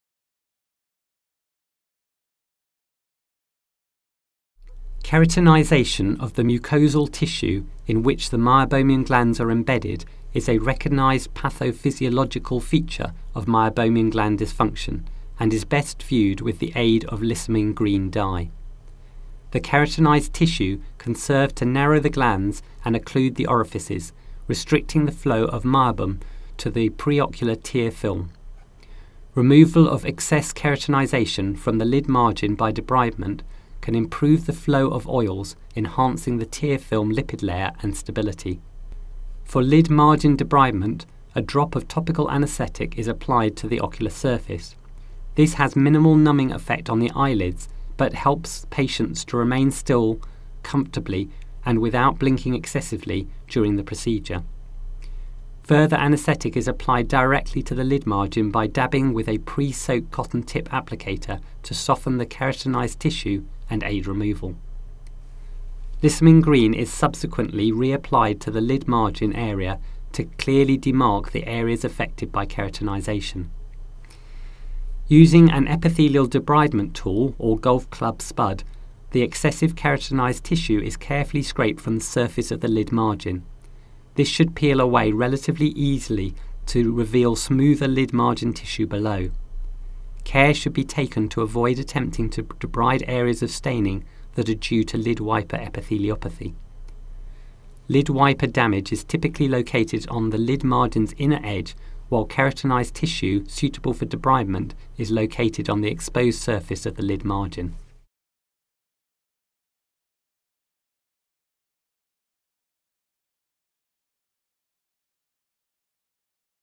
TFOS DEWS II Therapeutic Treatment Videos - Debridement
Debridement (removal) of keratinised tissue along the exposed lid margin to encourage release of meibum from the meibomian gland orifices into the tear film.